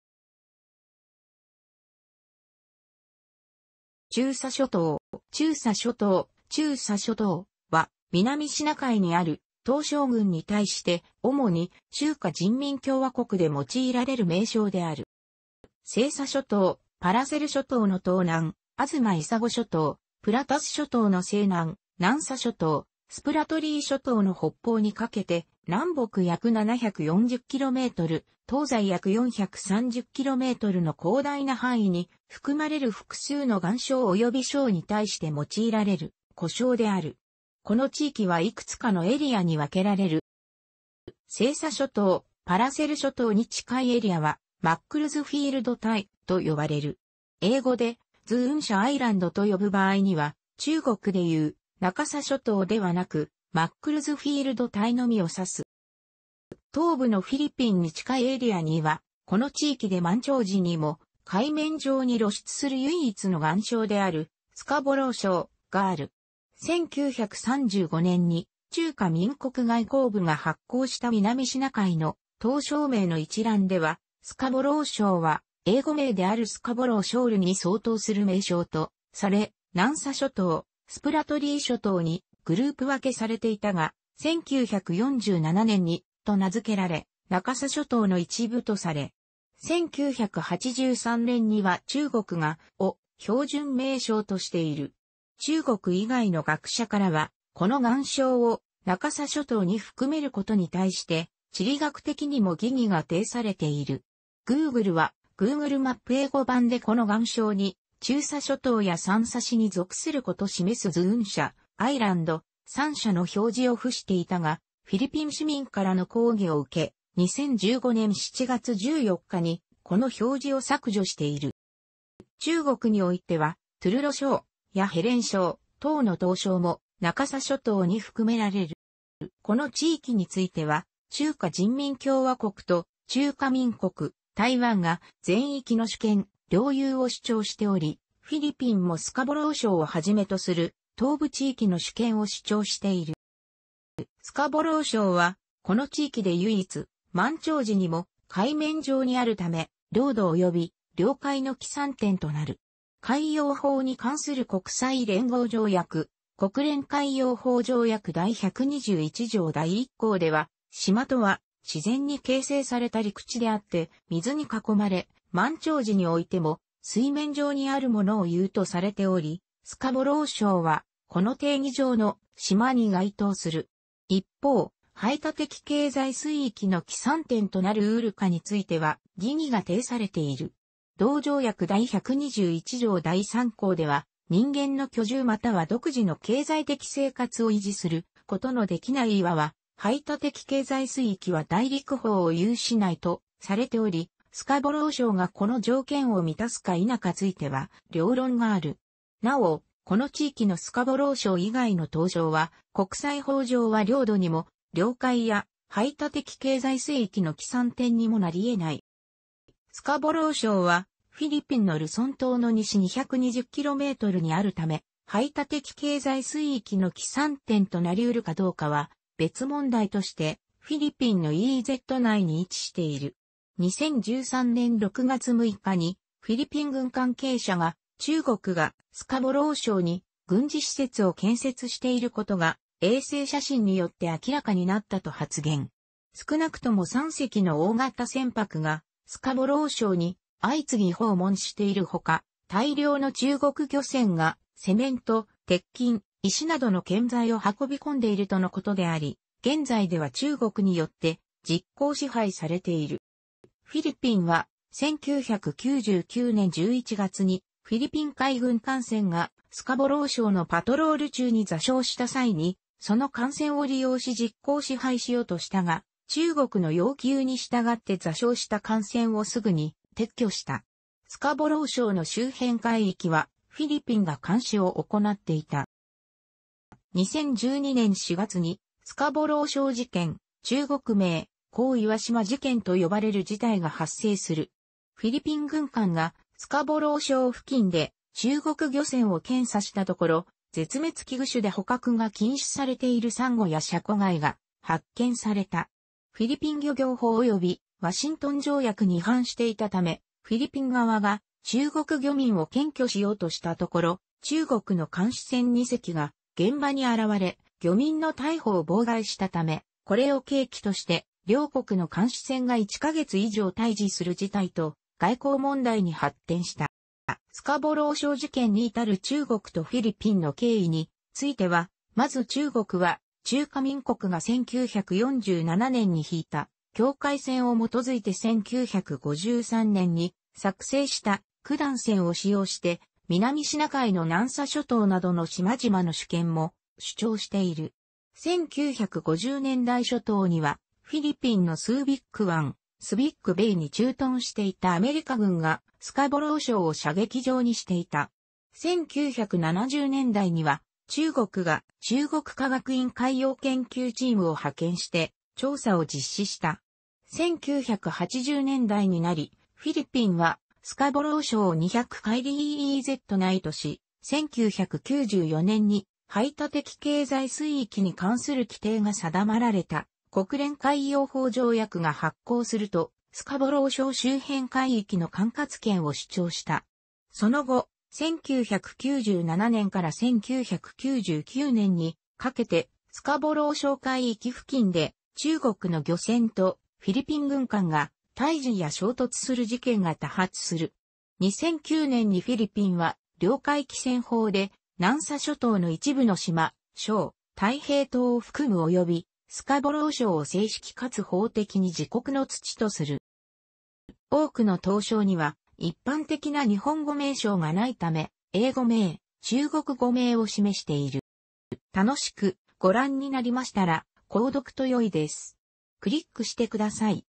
中沙諸島
中沙諸島（ちゅうさしょとう、（）、、）は、南シナ海にある島礁群に対して、主に中華人民共和国で用いられる名称である。

西沙諸島（パラセル諸島）の東南、東沙諸島（プラタス諸島）の西南、南沙諸島（スプラトリー諸島）の北方にかけて、南北約740km、東西約430kmの広大な範囲に含まれる複数の岩礁および環礁に対して用いられる呼称である。この地域はいくつかのエリアに分けられる。

西沙諸島（パラセル諸島）に近いエリアはマックルズフィールド堆（）と呼ばれる。英語で"Zhongsha Island"と呼ぶ場合には、中国でいう「中沙諸島」ではなく、マックルズフィールド堆のみを指す。

東部のフィリピンに近いエリアには、この地域で満潮時にも海面上に露出する唯一の岩礁であるスカボロー礁（、）がある。1935年に中華民国外交部が発行した南シナ海の島礁名の一覧では、スカボロー礁は英語名である"Scarborough Shoal"に相当する名称とされ、南沙諸島（スプラトリー諸島）にグループ分けされていたが、1947年に「」と名付けられ「中沙諸島」の一部とされた。1983年には中国が「」を標準名称としている。中国以外の学者からは、この岩礁を「中沙諸島」に含めることに対して地理学的にも疑義が呈されている。Googleは、Google マップ英語版でこの岩礁に中沙諸島や三沙市に属すること示す"Zhongsha Island, Sansha"の表示を付していたが、フィリピン市民からの抗議を受け、2015年7月14日にこの表示を削除している。

中国においては、トゥルロ礁（、）やヘレン礁（、）等の島礁も「中沙諸島」に含められる。

この地域については、中華人民共和国と中華民国（台湾）が全域の主権（領有）を主張しており、フィリピンもスカボロー礁をはじめとする東部地域の主権を主張している。

スカボロー礁は、この地域で唯一、満潮時にも海面上にあるため、領土および領海の起算点となる。「海洋法に関する国際連合条約（国連海洋法条約）」第121条第1項では、「島とは、自然に形成された陸地であって、水に囲まれ、満潮時においても水面上にあるものをいう」とされており、スカボロー礁はこの定義上の「島」に該当する。一方、排他的経済水域(EEZ)の起算点となる得るかについては疑義が呈されている。同条約第121条第3項では、「人間の居住又は独自の経済的生活を維持することのできない岩は､排他的経済水域又は大陸棚を有しない」とされており、スカボロー礁がこの条件を満たすか否かついては両論がある。なお、この地域のスカボロー礁以外の島礁は、国際法上は領土にも、領海や排他的経済水域の起算点にもなり得ない。

スカボロー礁は、フィリピンのルソン島の西220kmにあるため、排他的経済水域の起算点となり得るかどうかは別問題として、フィリピンのEEZ内に位置している。2013年6月6日にフィリピン軍関係者が、中国がスカボロー礁に軍事施設を建設していることが衛星写真によって明らかになったと発言。少なくとも3隻の大型船舶がスカボロー礁に相次ぎ訪問しているほか、大量の中国漁船がセメント、鉄筋、石などの建材を運び込んでいるとのことであり、現在では中国によって実効支配されている。

フィリピンは、1999年11月にフィリピン海軍艦船がスカボロー礁のパトロール中に座礁した際に、その艦船を利用し実効支配しようとしたが、中国の要求に従って座礁した艦船をすぐに撤去した。スカボロー礁の周辺海域は、フィリピンが監視を行っていた。

2012年4月にスカボロー礁事件（中国名: 黄岩島事件）と呼ばれる事態が発生する。フィリピン軍艦が、スカボロー礁付近で中国漁船を検査したところ、絶滅危惧種で捕獲が禁止されているサンゴやシャコ貝が発見された。フィリピン漁業法およびワシントン条約に違反していたため、フィリピン側が中国漁民を検挙しようとしたところ、中国の監視船2隻が現場に現れ、漁民の逮捕を妨害したため、これを契機として両国の監視船が1か月以上対峙する事態と外交問題に発展した。

「スカボロー礁事件」に至る中国とフィリピンの経緯については、まず中国は、中華民国が1947年に引いた境界線を基づいて1953年に作成した「九段線」を使用して、南シナ海の南沙諸島などの島々の主権も主張している。1950年代初頭には、フィリピンのスービック湾（Su...